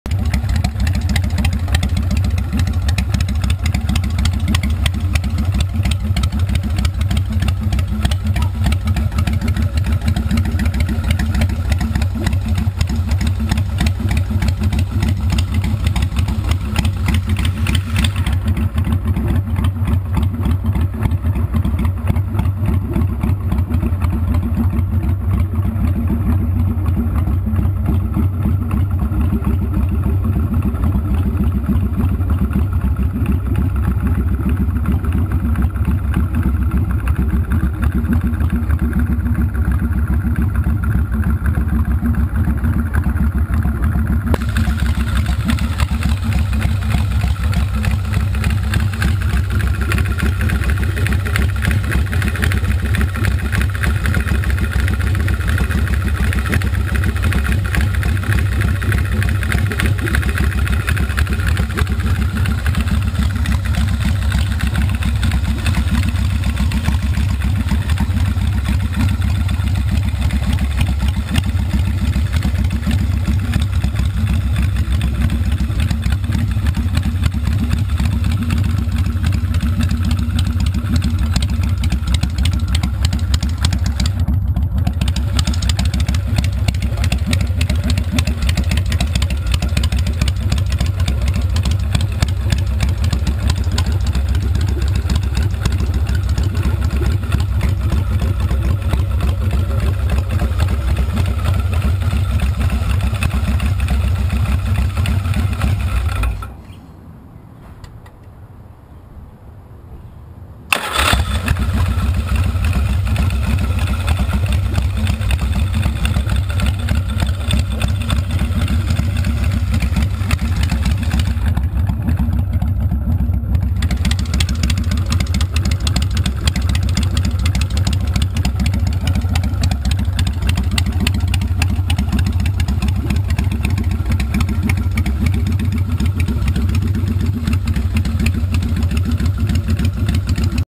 SoftailChopper/ Bobber for sale
SOFTAIL CHOPPER SHOWN RUNNING
CURRENTLY UP FOR SALE
PLEASE EXCUSE THE "FISH EYE" EFFECT